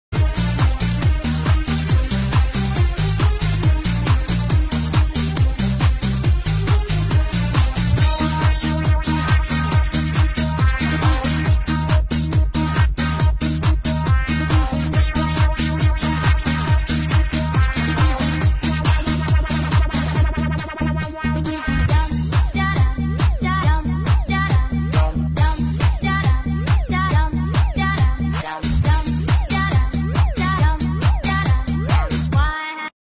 Stugna P - another shoot
My favourite weapon in Ukraine, Stugna P, made in UA, is hitting again.